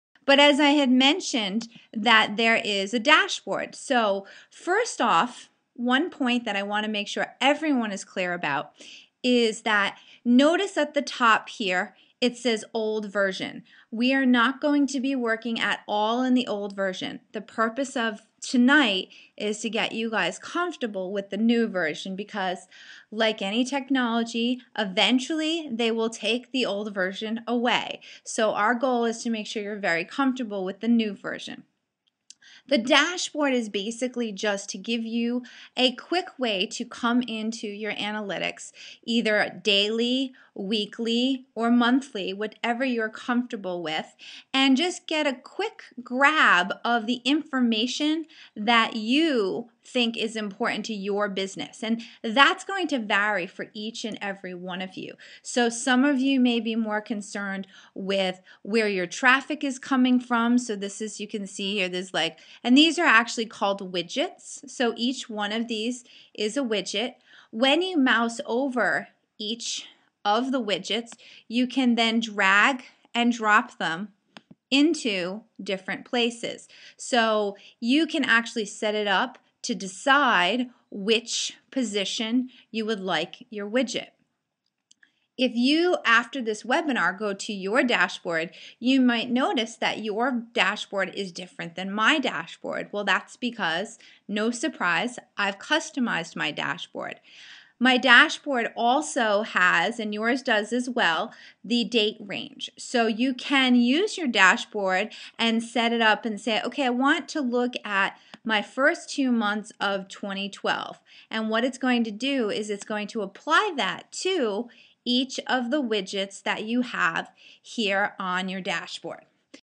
Google Analytics - Getting Started with Dashboard
Want to know what a webinar is like with Jennifer Shaheen the Technology Therapist? Here is an excerpt from "Converting Customers with the New Google Analytics" given March 27, 2012.